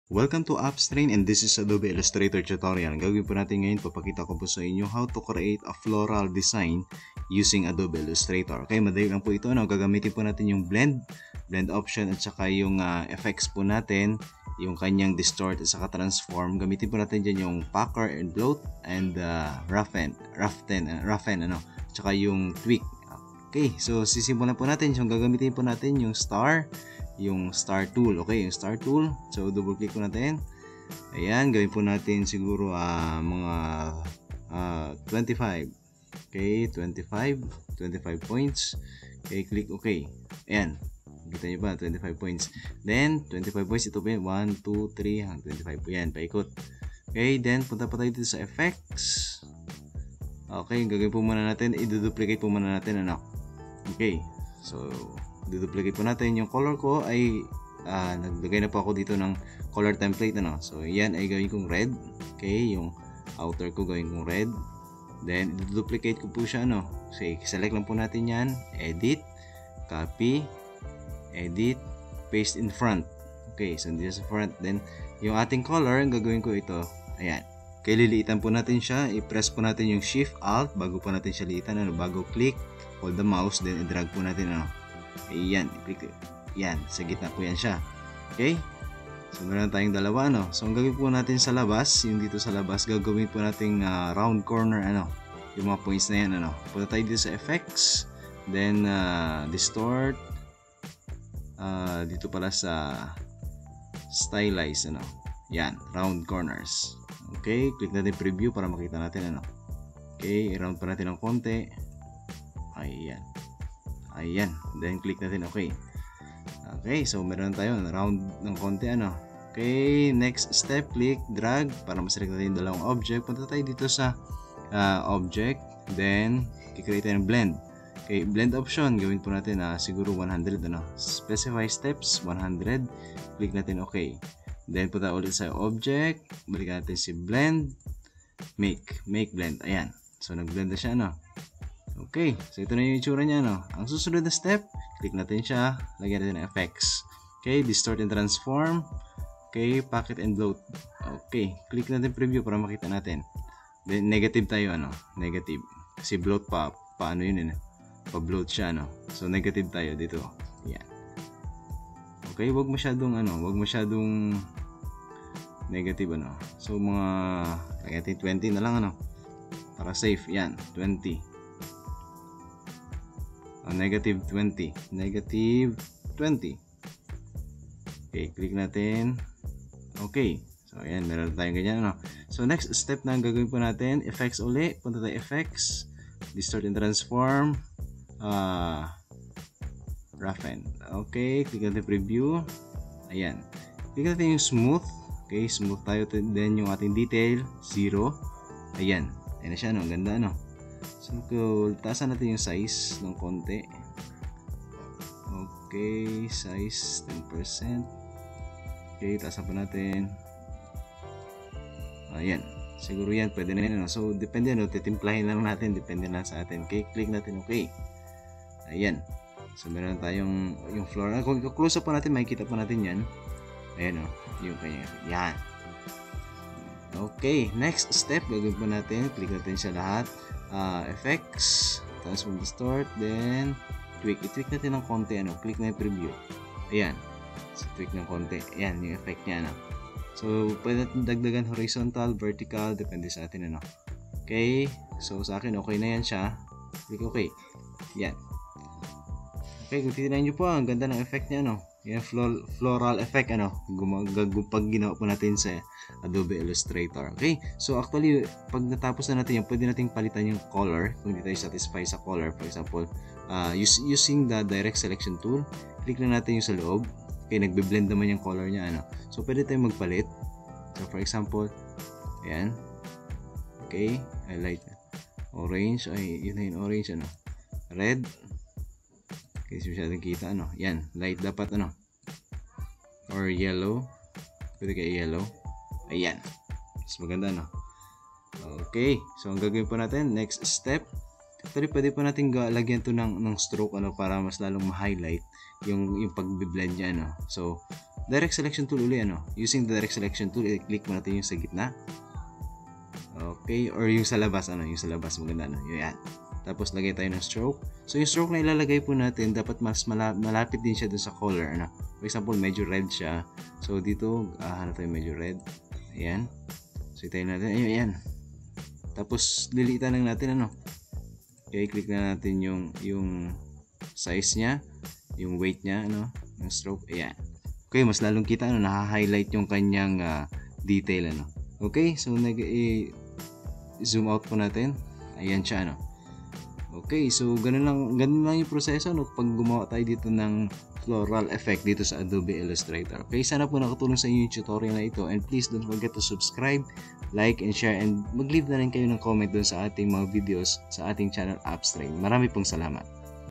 How to Create Floral Blend Design
In this How to Create Floral Blend Design tutorial we learn to create floral blend using blend and effects tool. Salamat po.